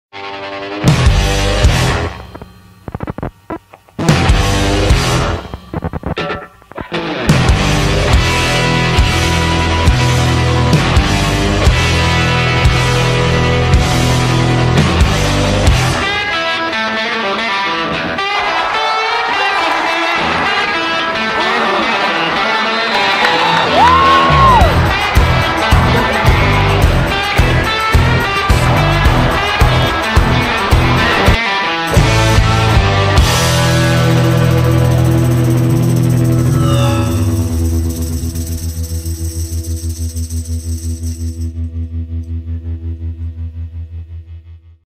Football Returns Promo
Put together a little promo for the return of Delaware Football on Sept 1, 2016.